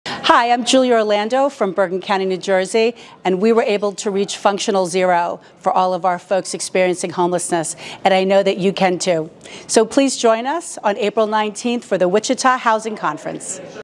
Join Continuum of Care and Julia Orlando for the Wichita Housing Conference April 29, 2023
Save the date! April 19, 2023
As part of the Wichita Housing Conference, our Continuum of Care team will be providing insights on state of homelessness in Wichita and bringing in housing advocate Julia Orlando to talk about the functional zero model.